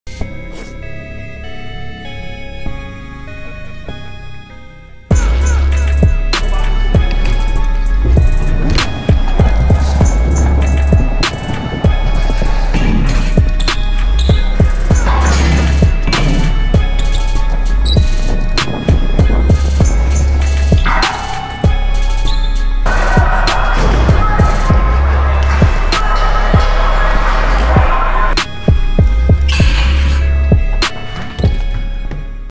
FourClipFriday Justin Hughes Vol II
F**k Mondays. Justin Hughes with some Canadian fire from Le Taz in Montreal. Shout out to OGC BMX holding it down over there.